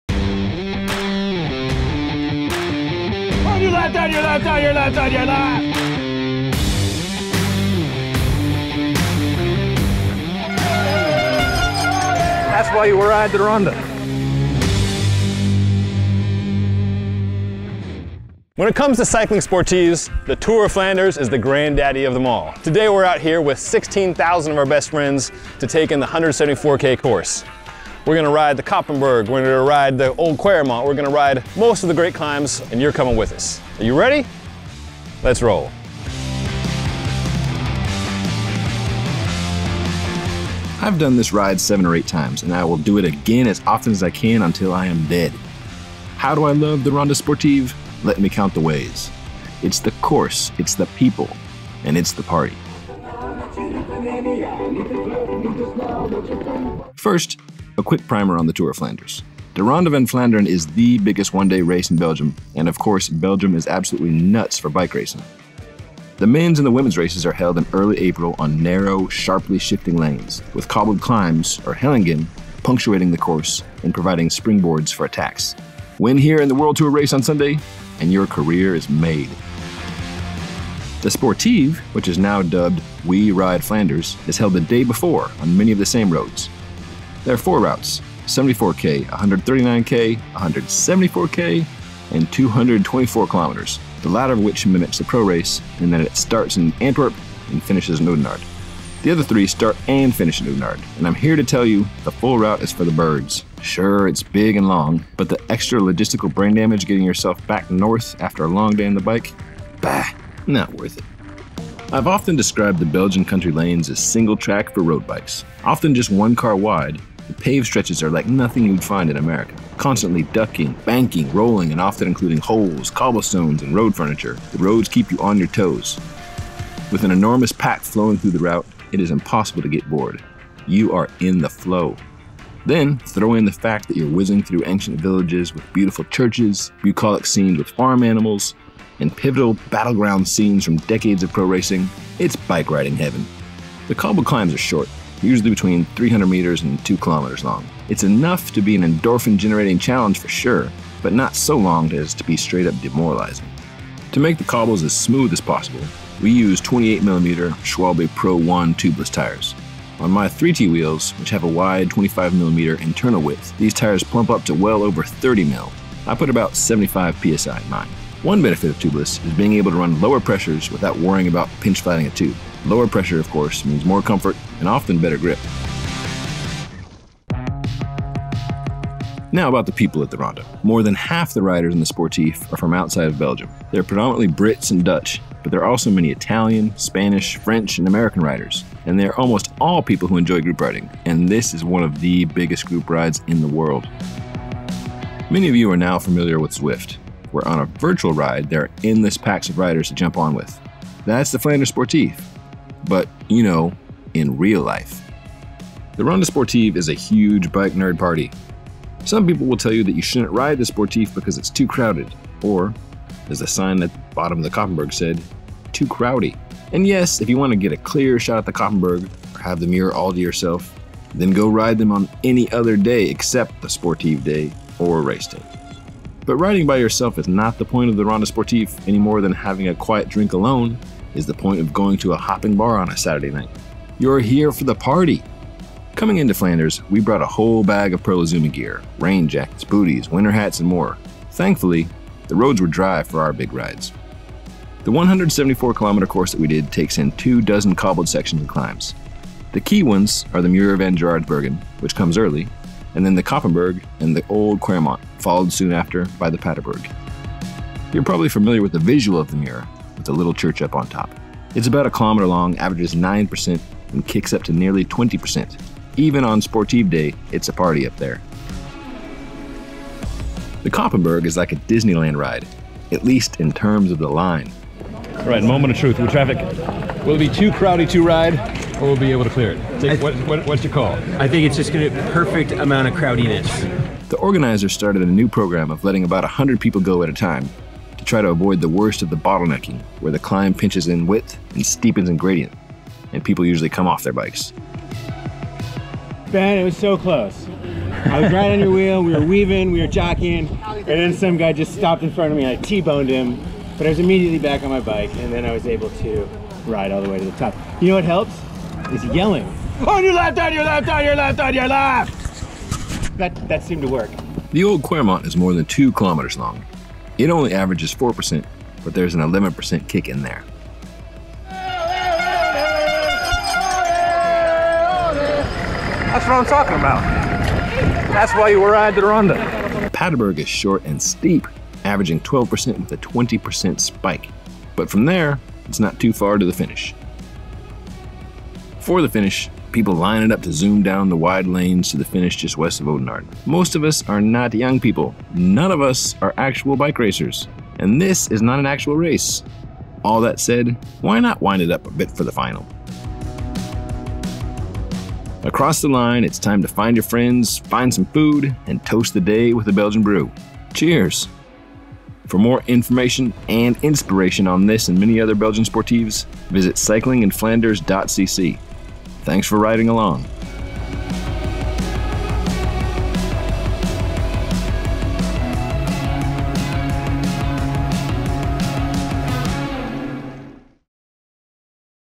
Riding the Tour of Flanders sportive
Want to ride the course of De Ronde van Vlaanderen with 16,000 friends? Then check out the 'We Ride Flanders' sportive, held the day before the WorldTour race. Roll Massif's Ben Delaney and VeloNews editor jumped into the fray again this year. Come along for the ride!